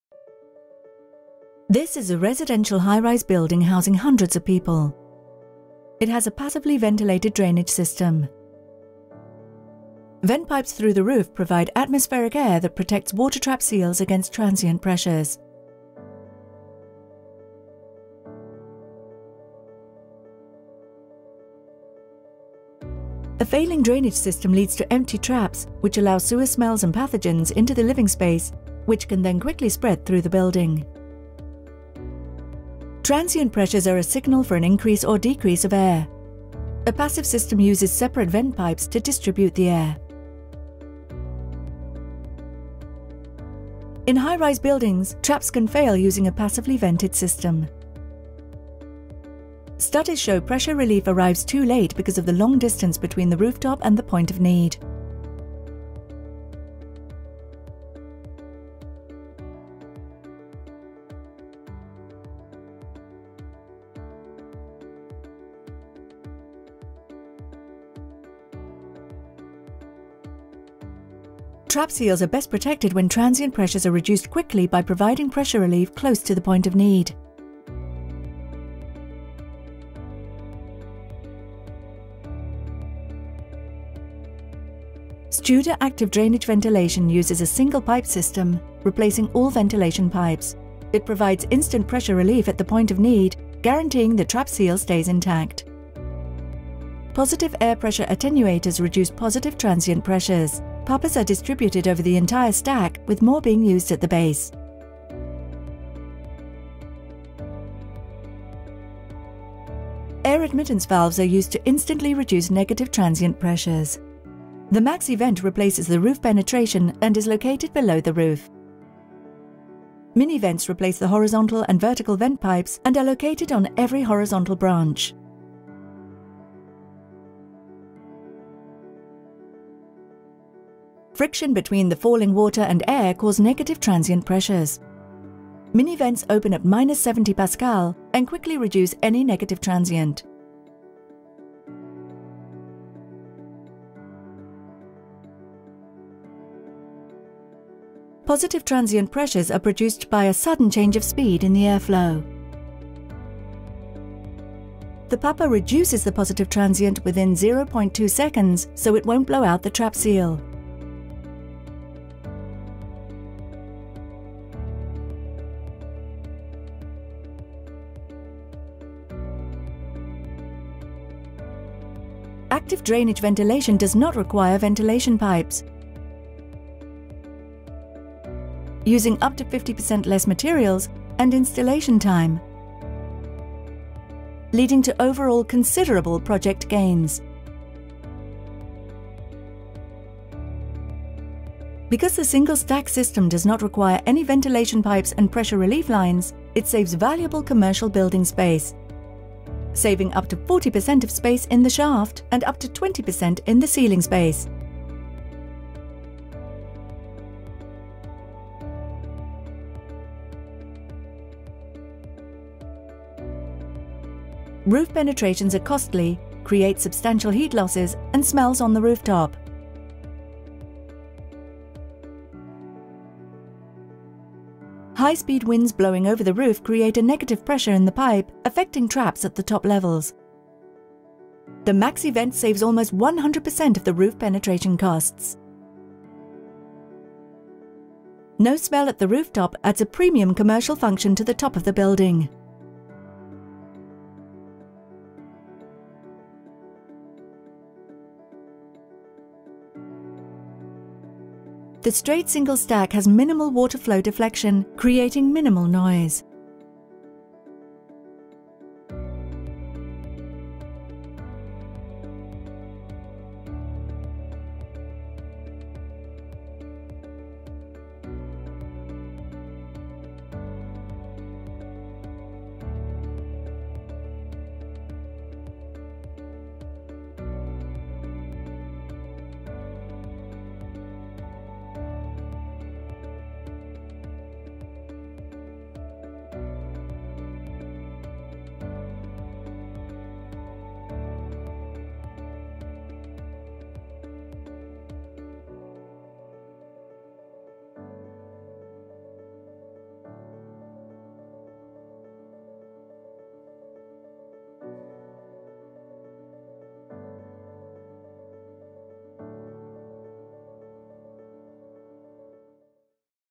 AU – Studor Active Drainage Ventilation
Drainage design of soil and waste systems within domestic and commercial buildings is of high importance. Studor has an innovative range of products to suit any architectural design, whether for a new or existing building.

The concept is simple; by providing plumbing ventilation within drainage systems, Studor products will prevent the loss of water seals in traps, and hence bad odours. From residential houses and high rise apartments to commercial towers, stadiums, hotels and some of the tallest buildings in the world, Studor has been leading the way with patented technology for drainage ventilation products since 1975.